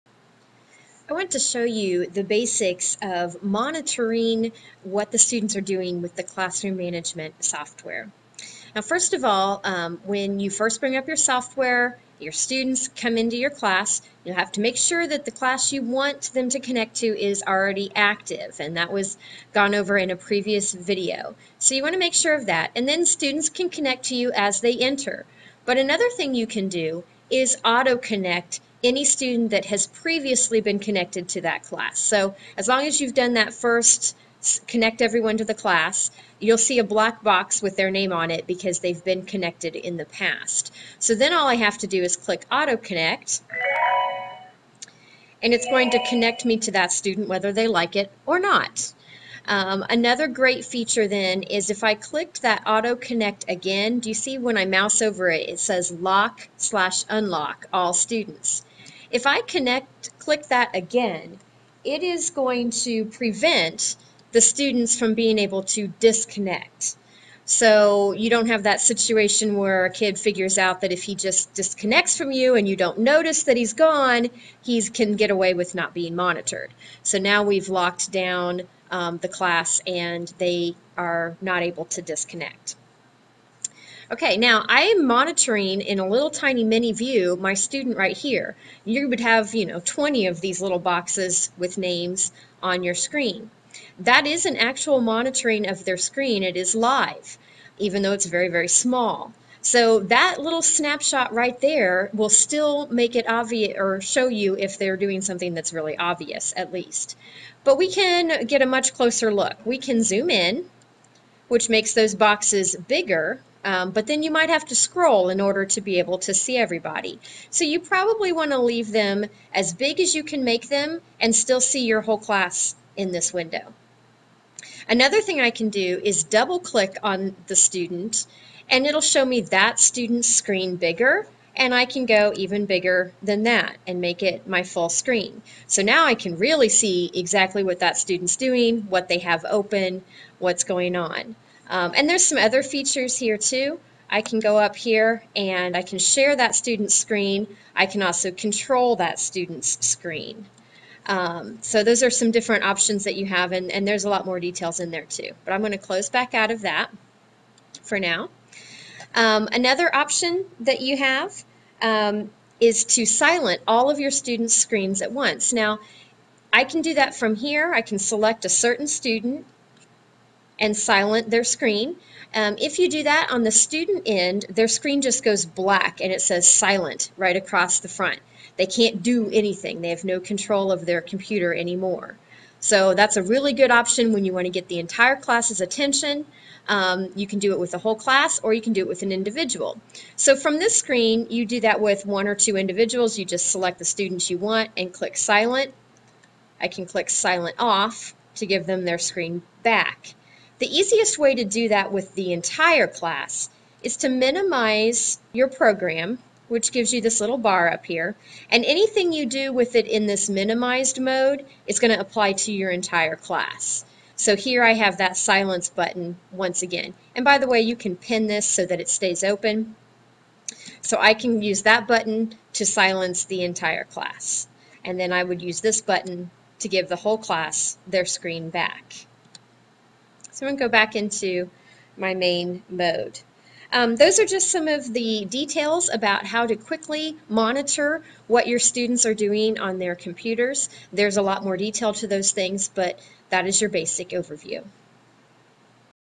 Monitoring Students
Video tutorial for the basics of how to monitor students with Classroom Management software on the DakTech PlaidLet.